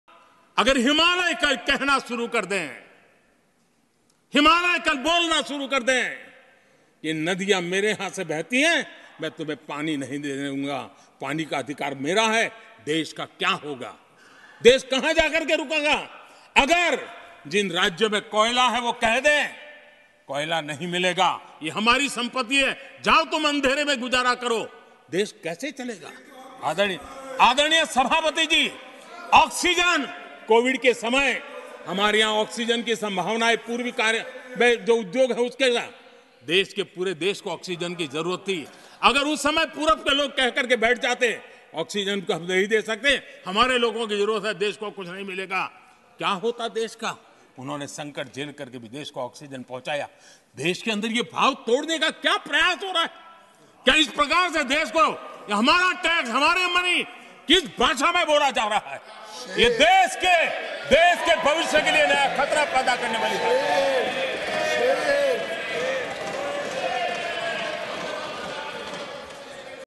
ಕರ್ನಾಟಕದ ವಿರುದ್ಧ ಮೋದಿ ಗುಡುಗು ..!
ನಮ್ಮ ತೆರಿಗೆ ಪಾಲು ಕೇಳಿದ್ದಕ್ಕೆ ಕುಚೋದ್ಯ..!